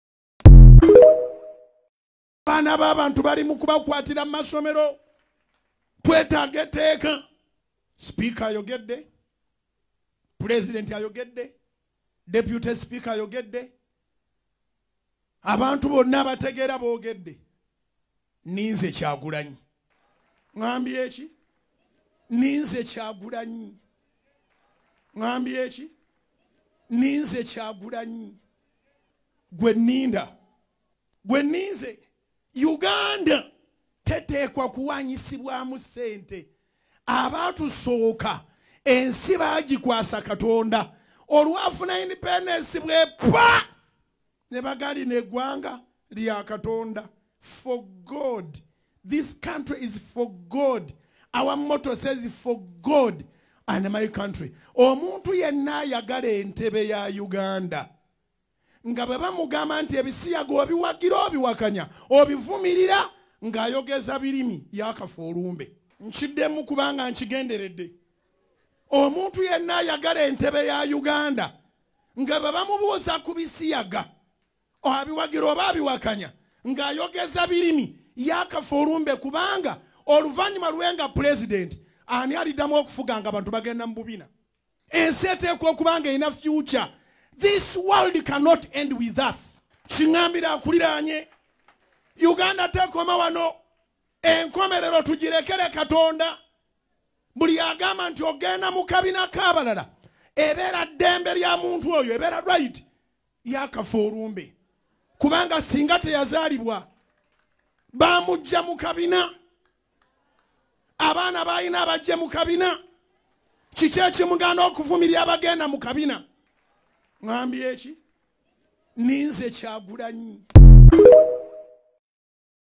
''Nze njagala BOBI WINE aveeyo avumirire ebisiyaga OBA abiwagire!" - Pr. Bujingo
Pastor Bujingo ayagala President wa National Unity platform aveeyo ayogere avumirire ebisiyaga OBA Aveeyo abaiwagire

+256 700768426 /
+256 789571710